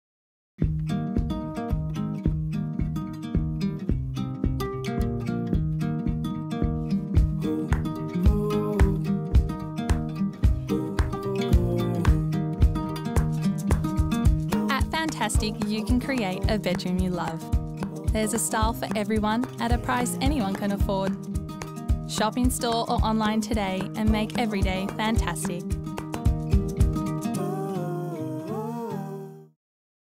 Fantastic Furniture TV Commercial - Bedroom
At Fantastic you can create a home you love!
There’s a style for everyone, at a price anyone can afford
So shop in store or online today and make every day Fantastic!